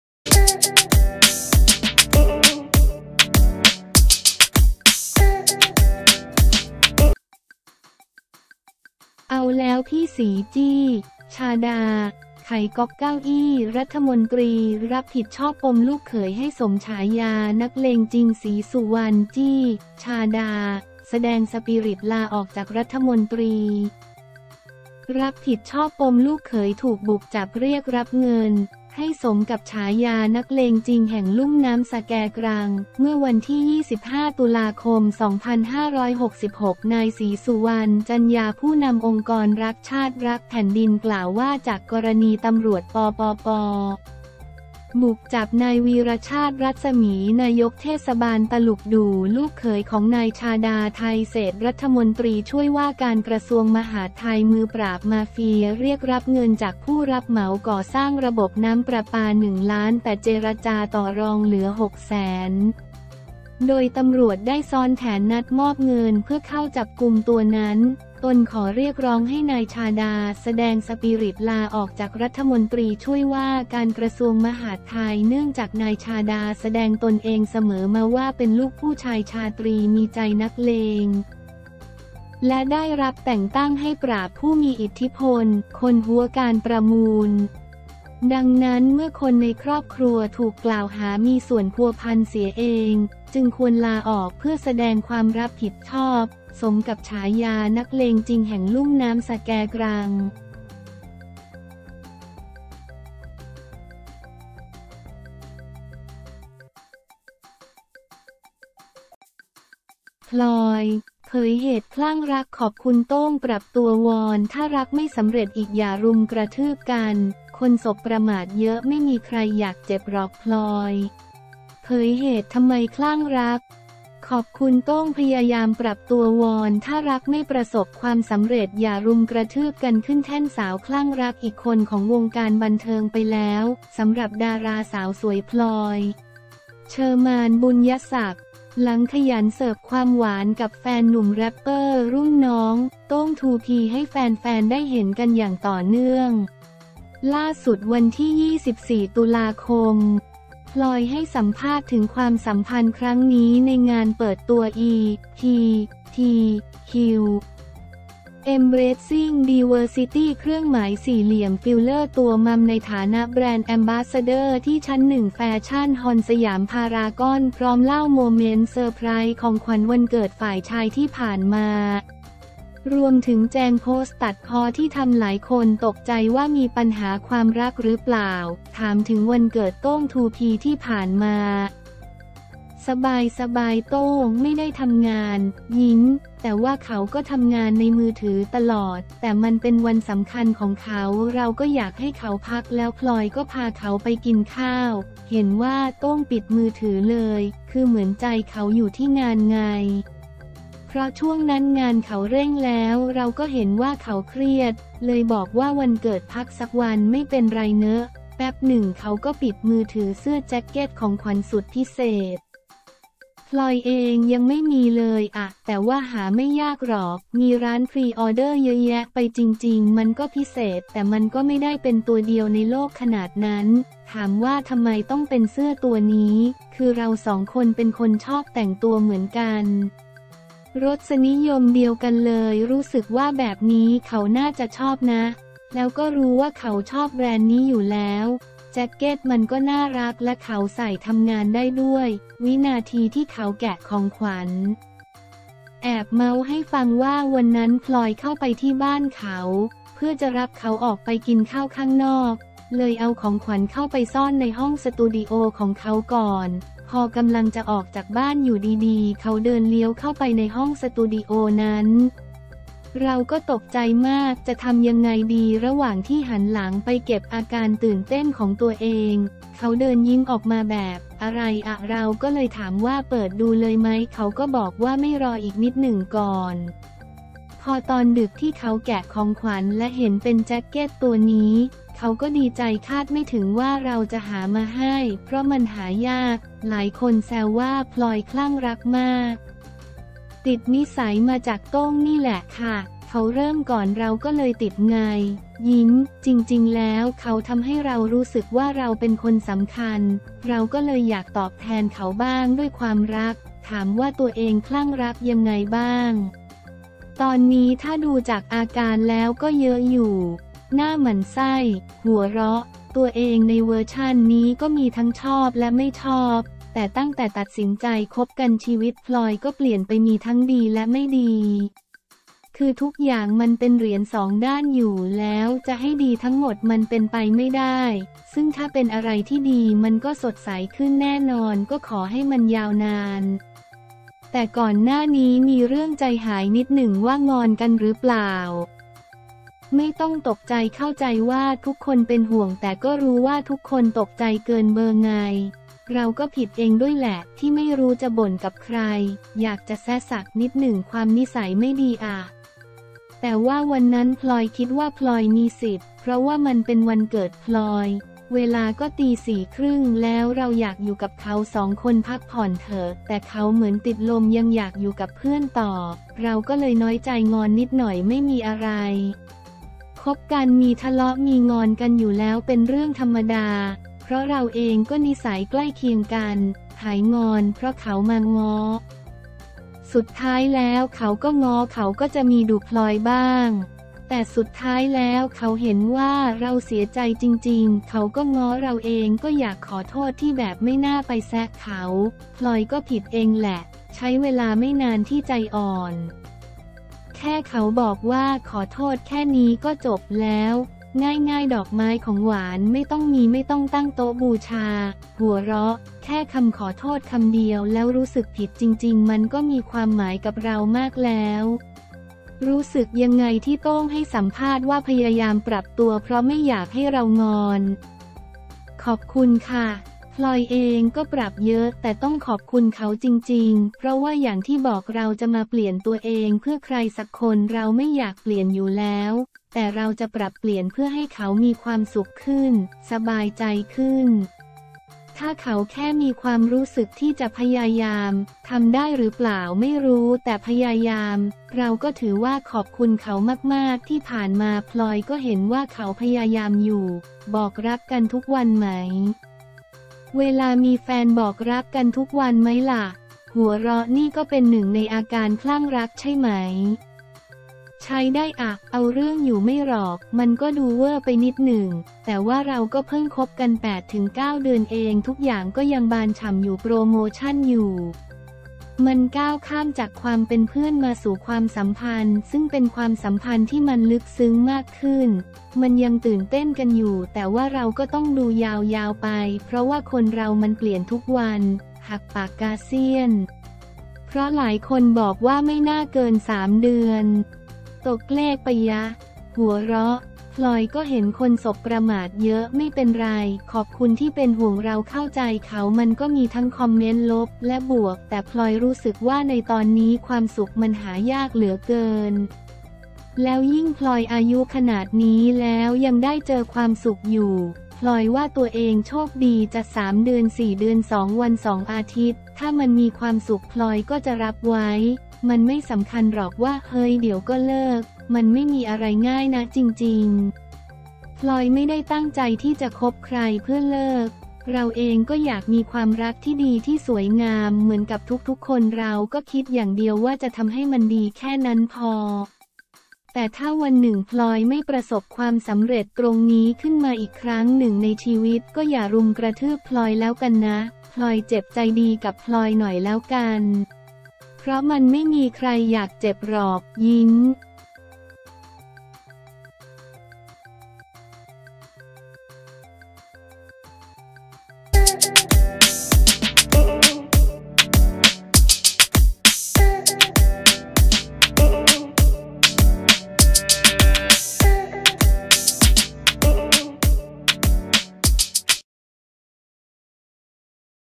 เอาแล้ว พี่ศรี จี้ ‘ชาดา’ ไขก๊อกเก้าอี้รมต รับผิดชอบปมลูกเขย ให้สมฉายานักเลงจริง
ศรีสุวรรณ จี้ ‘ชาดา’ แสดงสปิริตลาออกจากรมต. รับผิดชอบ ปมลูกเขยถูกบุกจับเรียกรับเงิน ให้สมกับฉายานักเลงจริงแห่งลุ่มน้ำสะแกกรัง
เมื่อวันที่ 25 ต.ค.2566 นายศรีสุวรรณ จรรยา ผู้นำองค์กรรักชาติ รักแผ่นดิน กล่าวว่า จากกรณี ตำรวจ ปปป. บุกจับนายวีระชาติ รัศมี นายกเทศบาล ตลุกดู่ ลูกเขยของนายชาดา ไทยเศรษฐ์ รมช.มหาดไทย มือปราบมาเฟีย เรียกรับเงินจากผู้รับเหมาก่อสร้างระบบน้ำประปา 1 ล้าน แต่เจรจาต่อรองเหลือ 6 แสน โดยตำรวจได้ซ้อนแผนนัดมอบเงินเพื่อเข้าจับกุมตัวนั้น ตนขอเรียกร้องให้ นายชาดา แสดงสปิริตลาออกจาก รมช.มหาดไทย

เนื่องจากนายชาดา แสดงตนเองเสมอมาว่า เป็นลูกผู้ชายชาตรี มีใจนักเลง และได้รับแต่งตั้งให้ปราบผู้มีอิทธิพล คนฮั้วการประมูล ดังนั้นเมื่อคนในครอบครัวถูกกล่าวหามีส่วนพัวพันเสียเอง จึงควรลาออกเพื่อแสดงความรับผิดชอบ สมกับฉายานักเลงจริงแห่งลุ่มน้ำสะแกกรัง